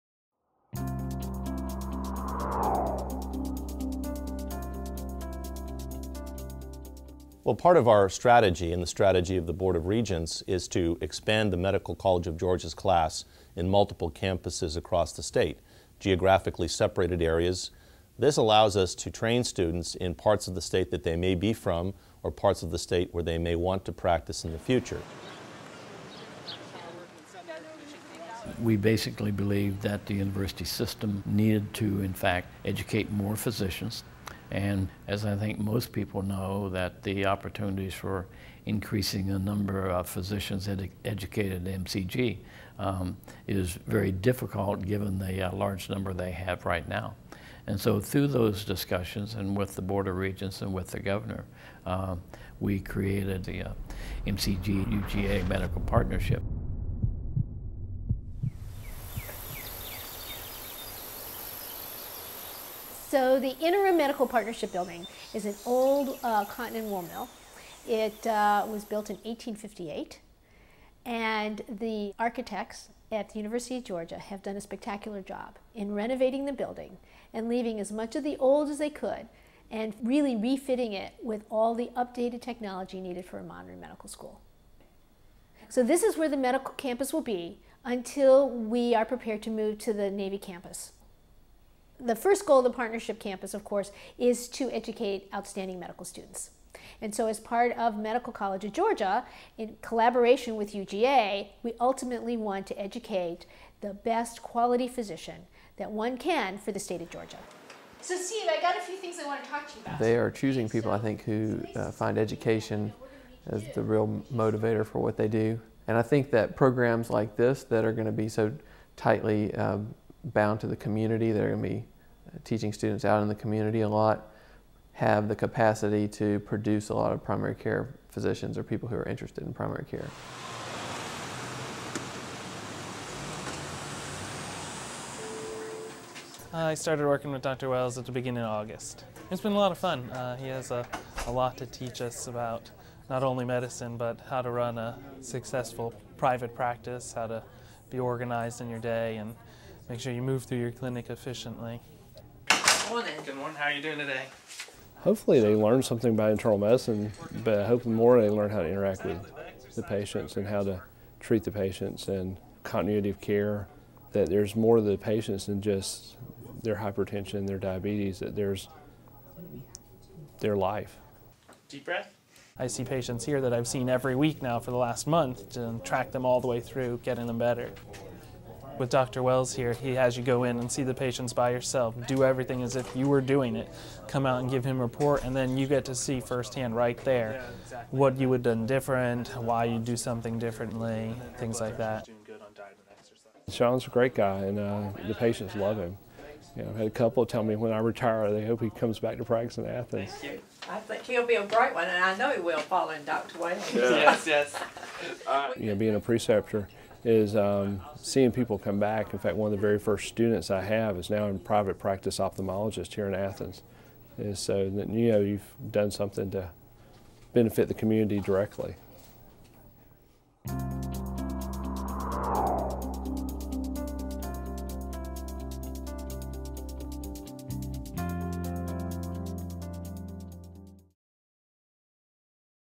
MCG/UGA Medical Partnership
The Medical College of Georgia and the University of Georgia have partnered to create a four-year medical education program in Athens to help alleviate a statewide shortage of physicians that threatens the health of Georgians. The MCG/UGA Medical Partnership combines the significant instructional and research resources of UGA, the states flagship land-grant research university, with the expertise of MCG, Georgias only public medical school.